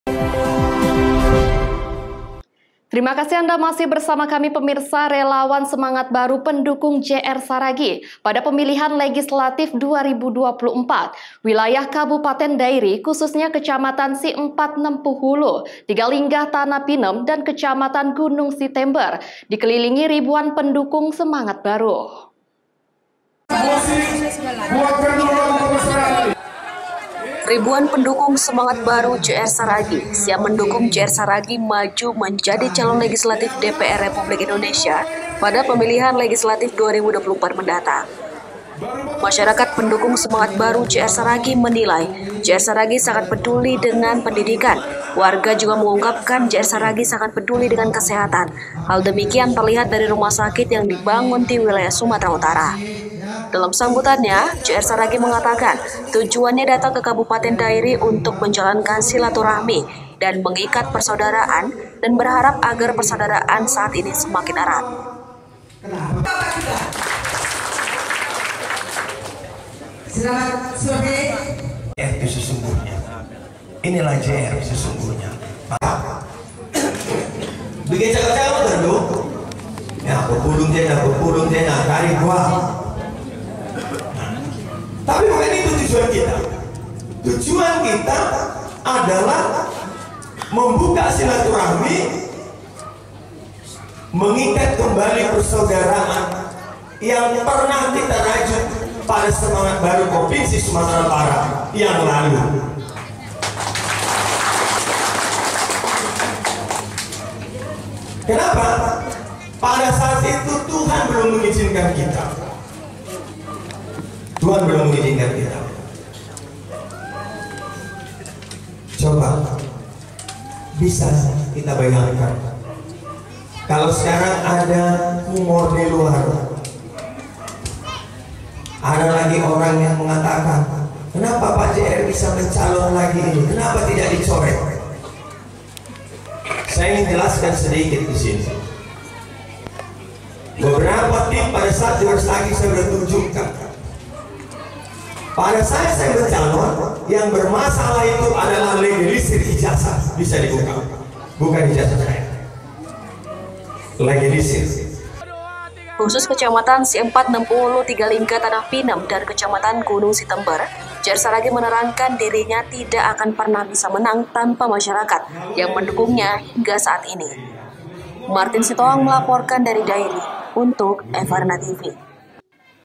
JR Saragih Disambut Relawan Tim Semangat Baru Dairi
Relawan semangat baru pendukung JR Saragih, pada pemilihan legislatif 2024, wilayah Kabupaten Dairi, khususnya Kecamatan Siempat Nempu Hulu, Tigalingga, Tanah Pinem, dan Kecamatan Gunung Sitembar. Dikelilingi ribuan pendukung semangat baru.